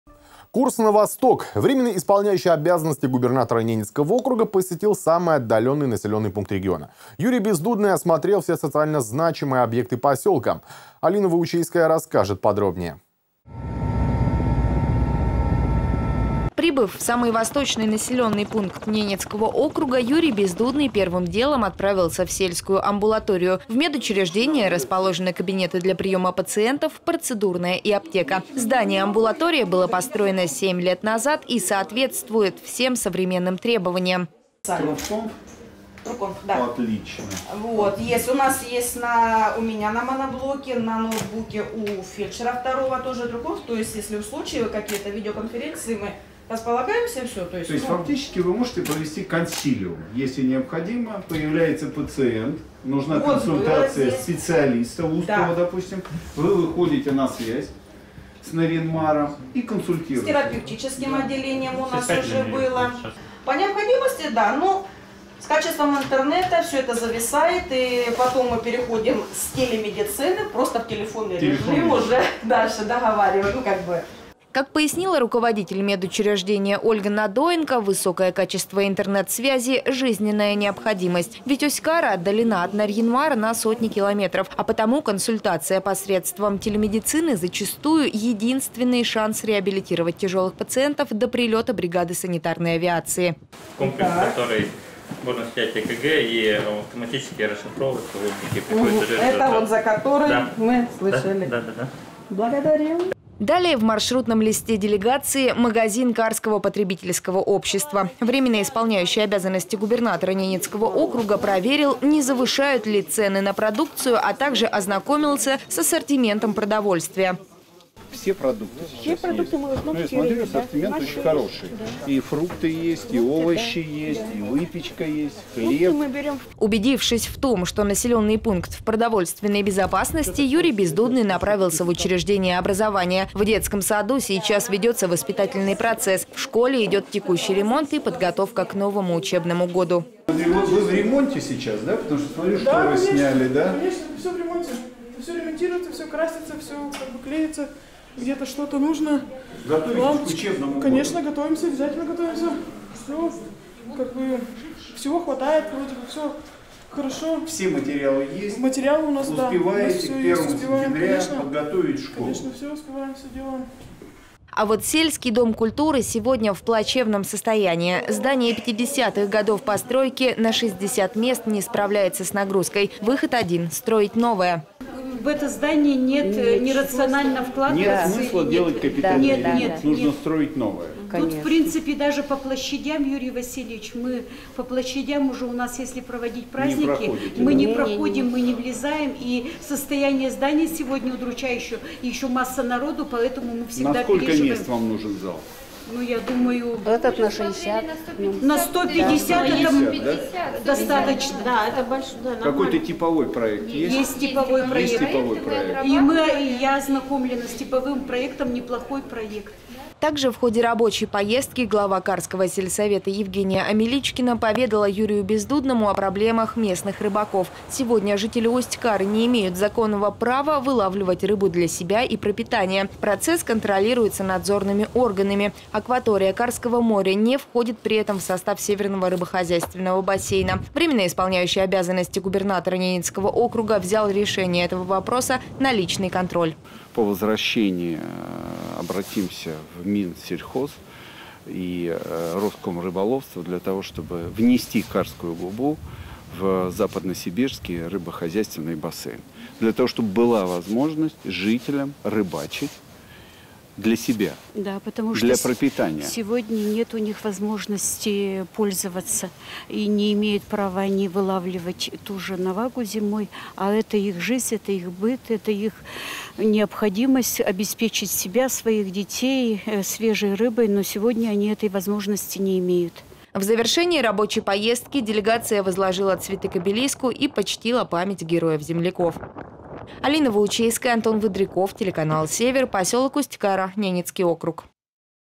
КУРС НА УСТЬ-КАРУ
Курс на Восток. Временно исполняющий обязанности губернатора Ненецкого автономного округа посетил самый отдаленный населенный пункт региона. Юрий Бездудный осмотрел все социально значимые объекты поселка.